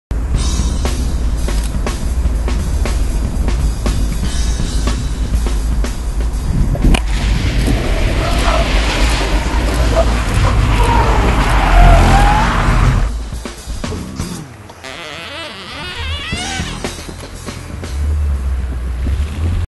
Rio rotiert
Drehungen mit dem Auto um die eigene Achse, sind eine heftige Angelegenheit. Durch ESP sind die Situationen seltener geworden. Auf Autobahnen sind aber immer wieder Spuren von solchen Drehungen zu sehen.